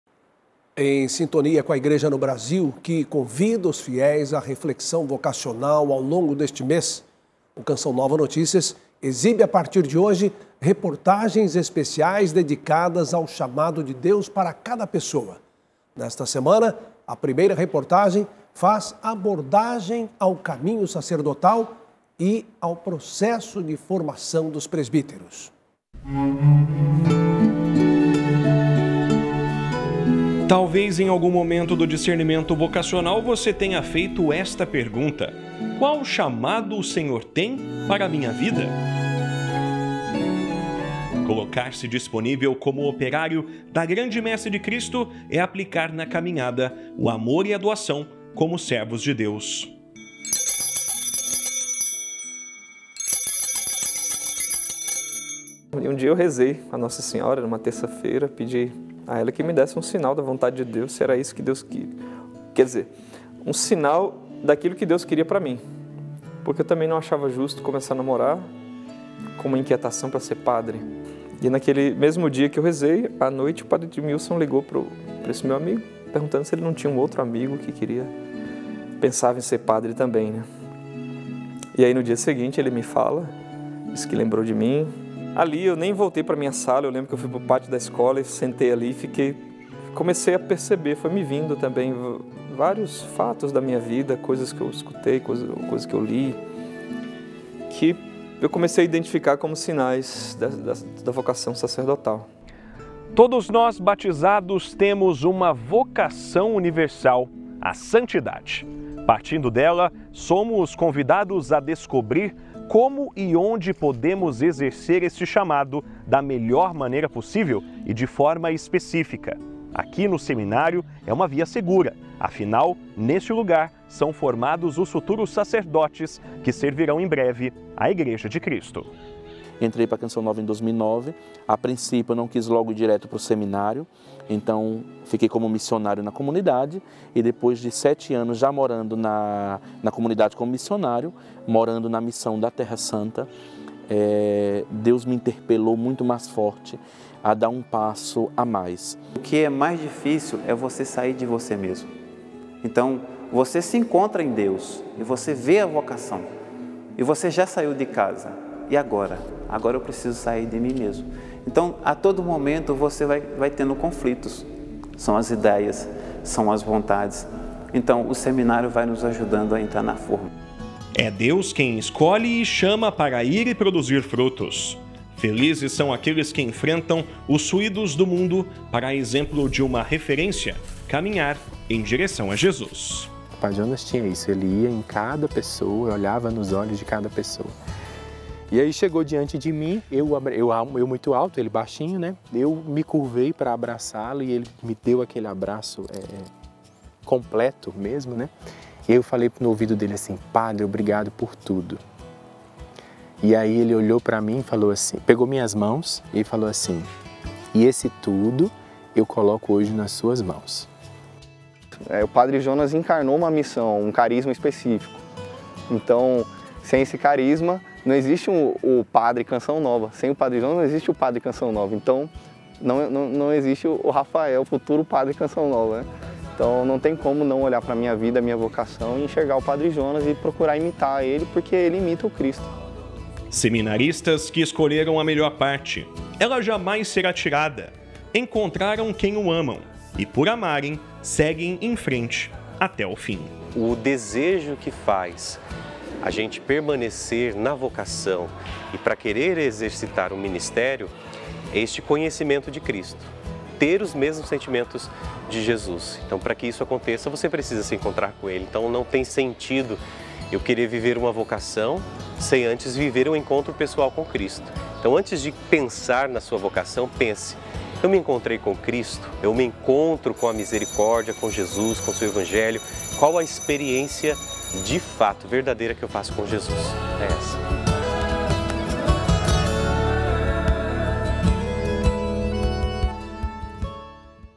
Reflexão vocacional: do caminho sacerdotal à formação de presbíteros
Em sintonia com a Igreja no Brasil, que convida à reflexão vocacional ao longo deste mês, o Canção Nova Notícias exibe, a partir desta quarta-feira, 7, reportagens especiais dedicadas ao chamado de Deus para cada pessoa. Nesta semana, a primeira reportagem faz abordagem ao caminho sacerdotal e o processo de formação dos presbíteros.

Reportagem de Emerson Tersigni e Ederaldo Paulini

Deus abençoe!